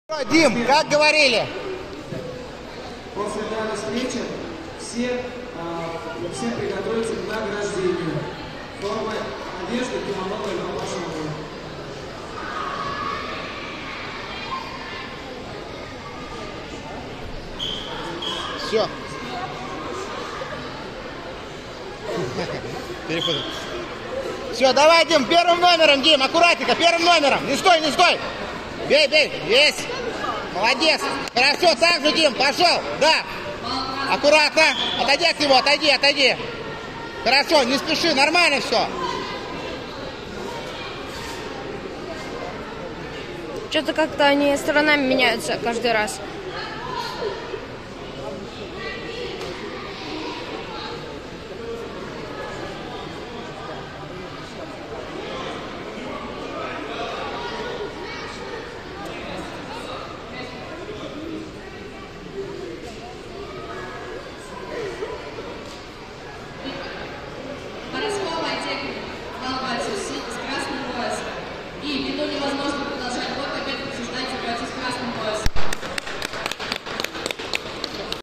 Петров Дмитрий (красный пояс), 1 бой, клуб ЯРОПОЛК, Турнир по Рукопашному бою, мкр. Митино, 06.11.22
6 ноября 2022 года в клубе Варяг в мкр. Митино прошёл Фестиваль рукопашного боя г. Москвы среди детей от 6 до 11 лет.
🥊 В турнире приняло участие более 123 спортсмена из 12 команд Москвы и Московской области.

2. Клуб в яндекс ДЗЕН
5. Резервный канал клуба в ЮТУБЕ (бои спортсменов клуба и т.д.- проф. и любительская видео съёмка)